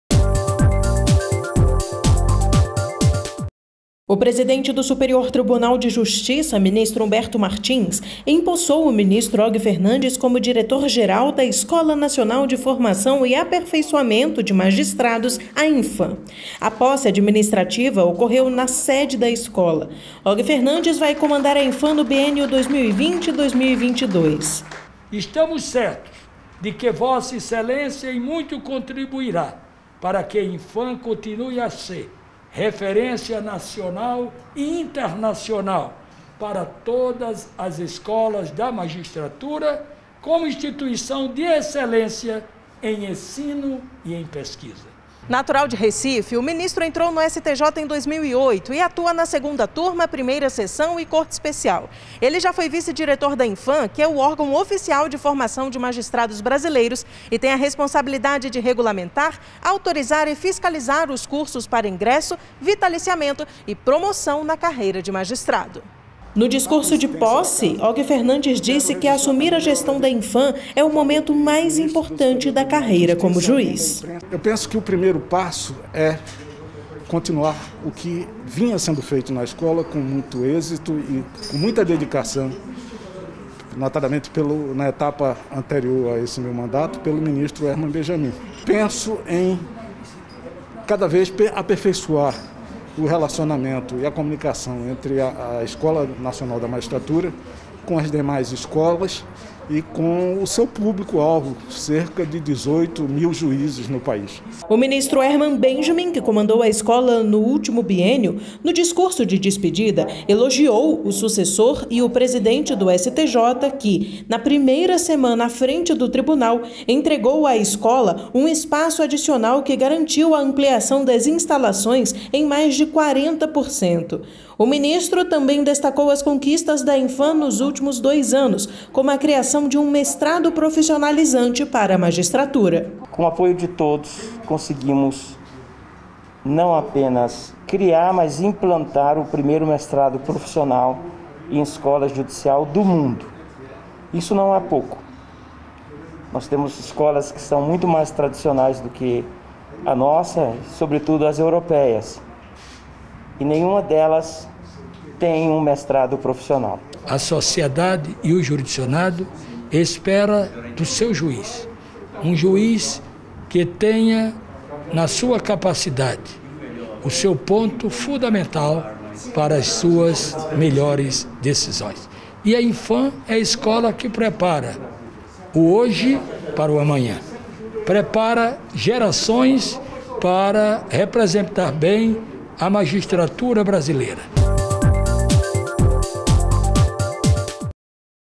Ministro Humberto Martins empossa ministro Og Fernandes na Enfam 04.09.20
O presidente do Superior Tribunal de Justiça (STJ), ministro Humberto Martins, empossou nesta quinta-feira (3) o ministro Og Fernandes no cargo de diretor-geral da Escola Nacional de Formação e Aperfeiçoamento de Magistrados (Enfam).